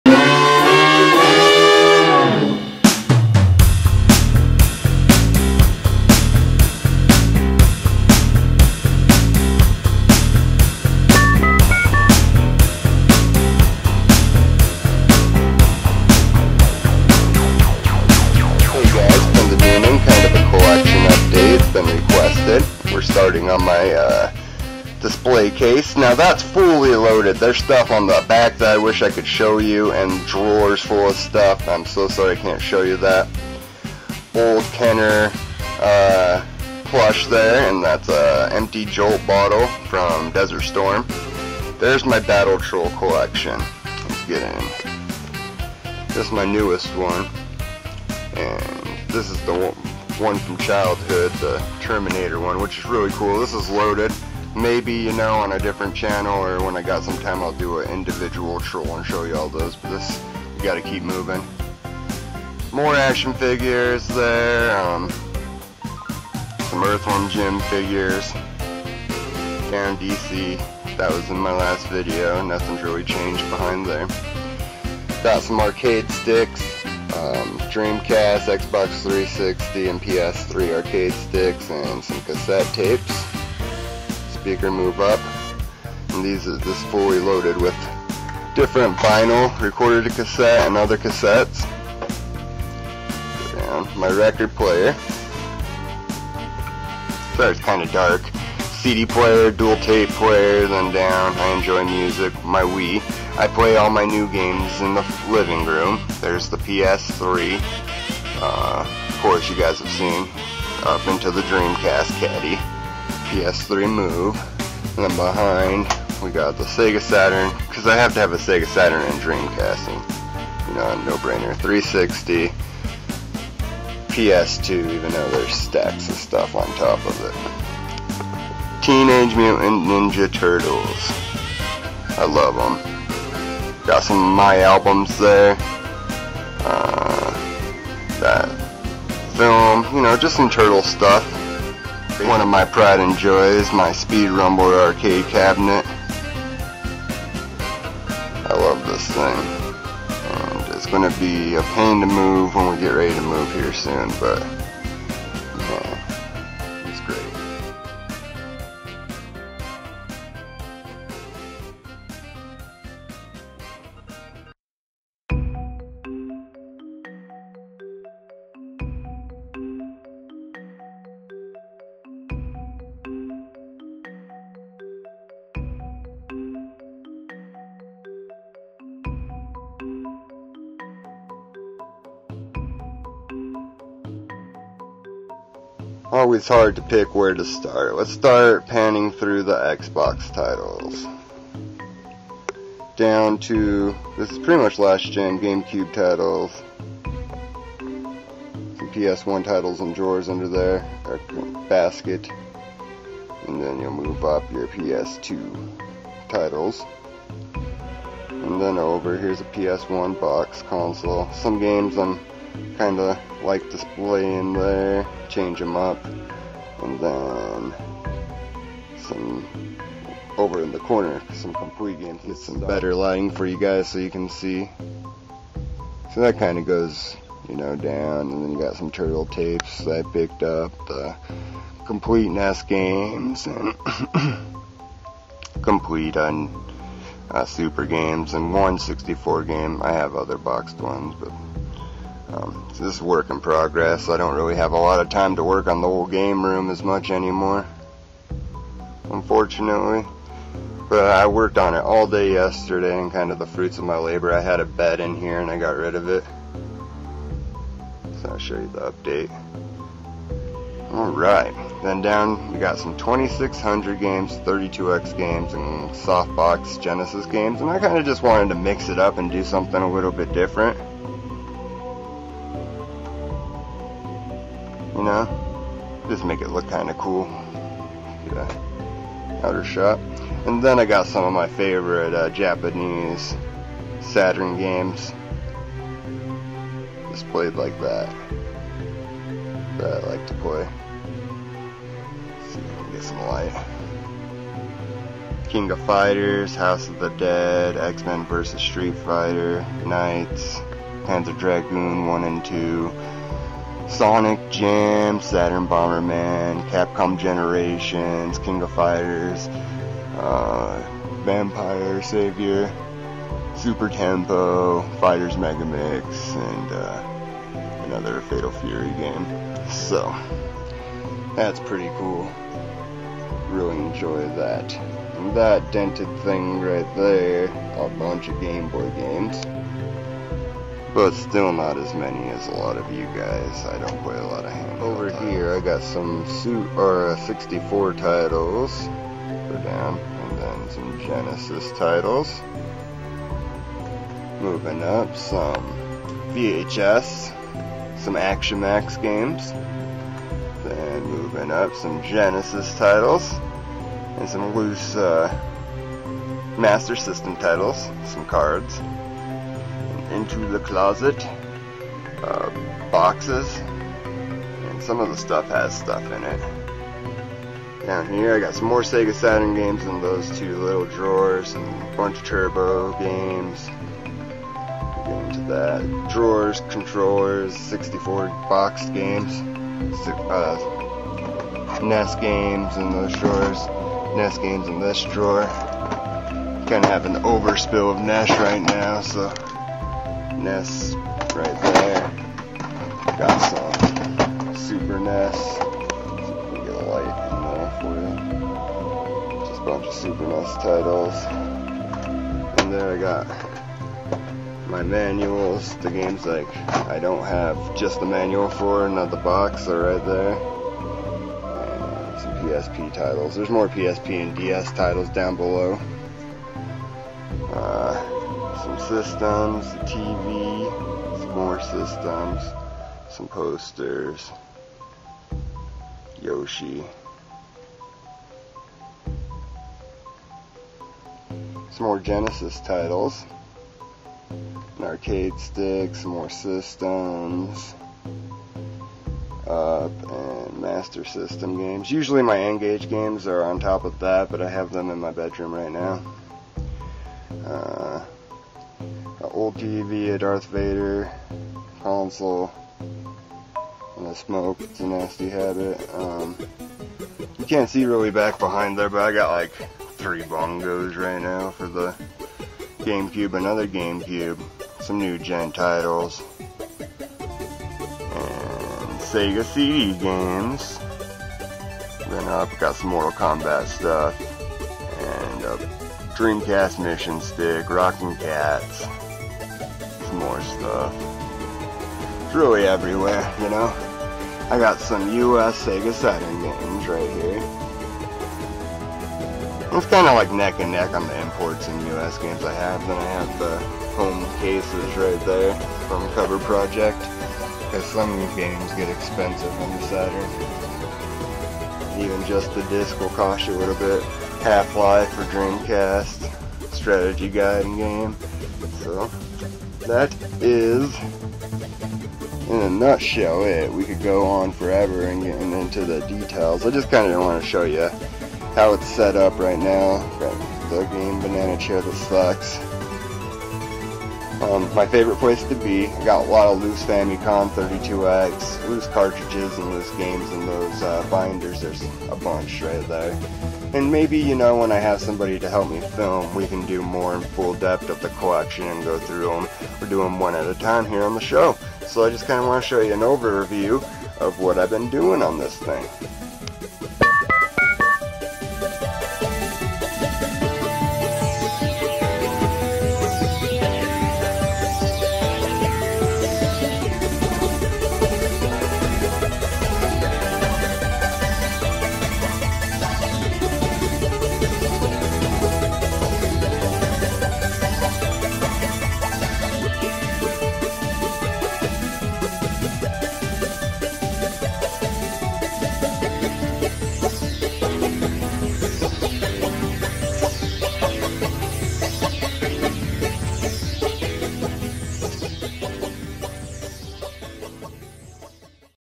Game Room Update 2K11 (Low Quality) Standard Definition
Hey Guys here's a quick update of my gameroom, I took out some furninture and rearranged.